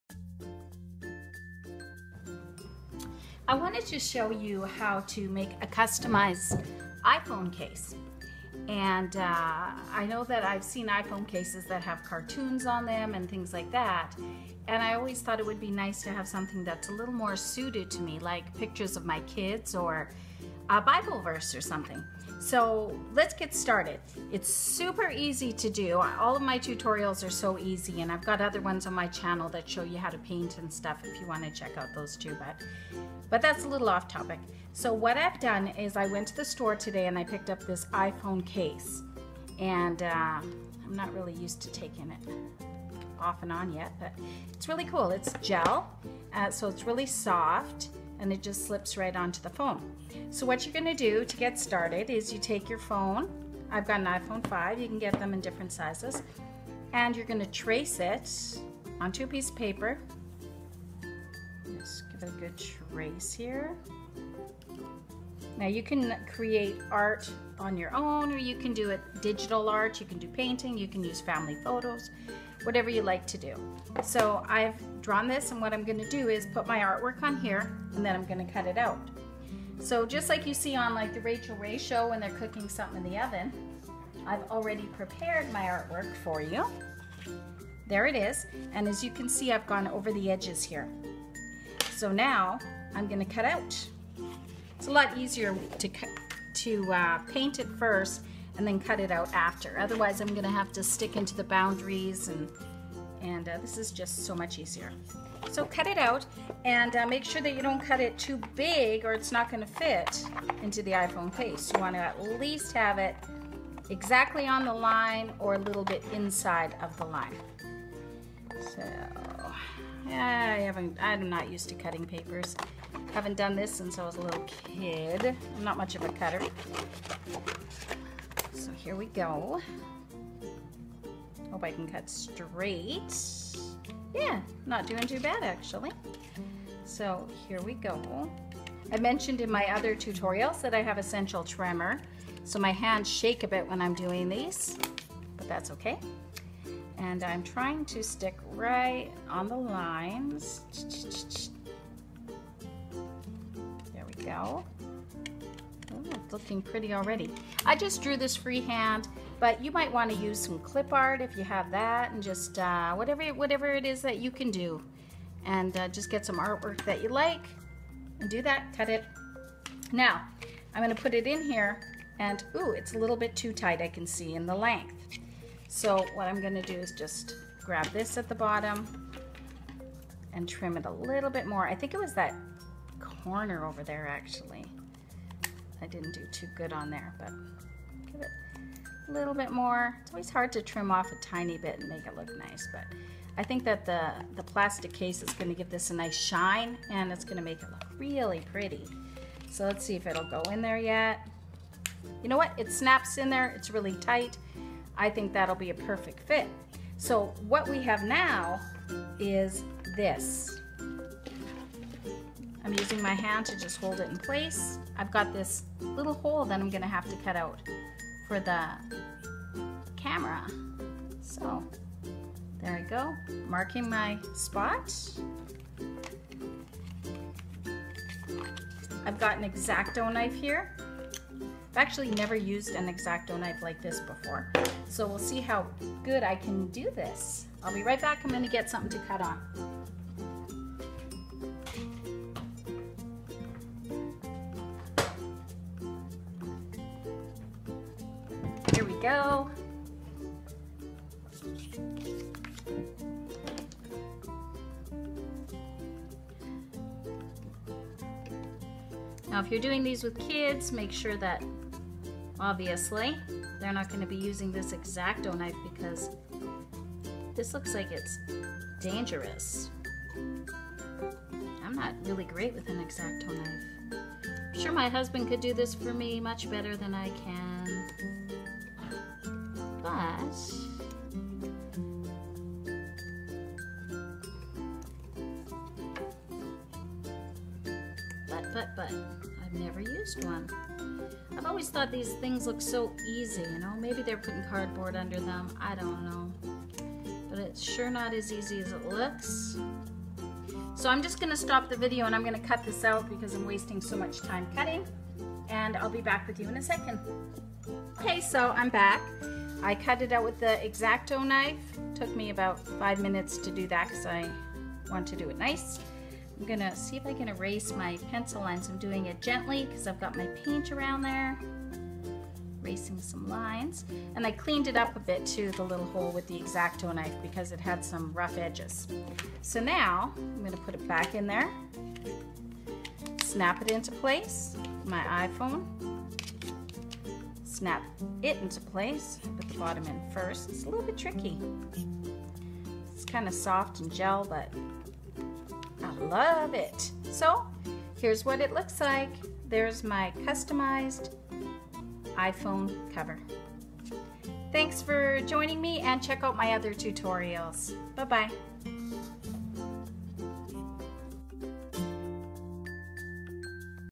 DIY Custom Iphone Cover
Tutorial for an easy to make, DIY iPhone Cover.